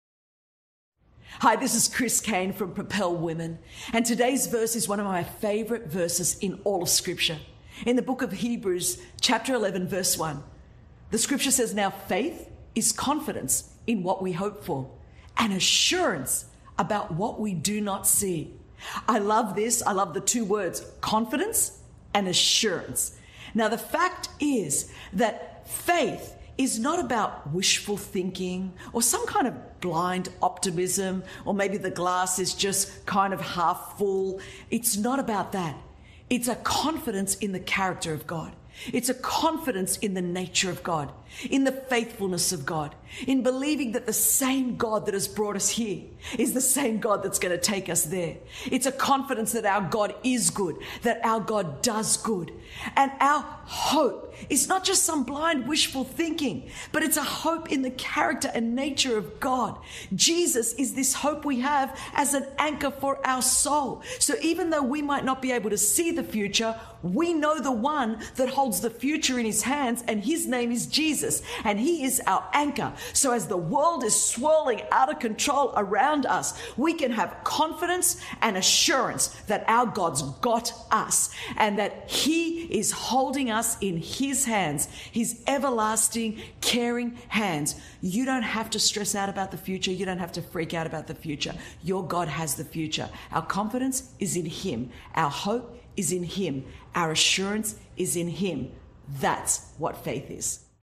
FAITH IS CONFIDENCE, HOPE AND ASSURANCE IN HIM...By Christine Caine... Hebrews 11:1
Now faith is the assurance (title deed, confirmation) of things hoped for (divinely guaranteed), and the evidence of things not seen [the conviction of their reality—faith comprehends as fact what cannot be experienced by the physical senses].
Hebrews 11:1 AMP